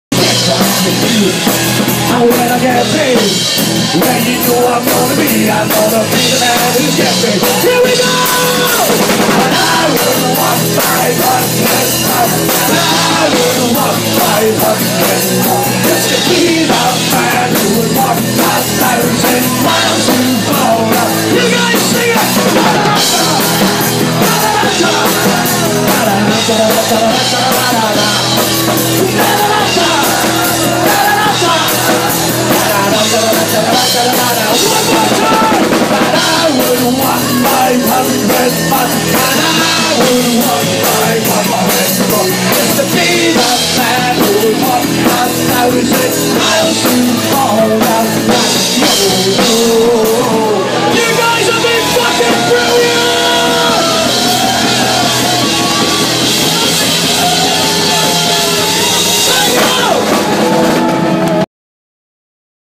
Ma Camerons in Aberdeen going crazy,Fubar
Ma Cameron's in Aberdeen packed and crazy with Fubar 2012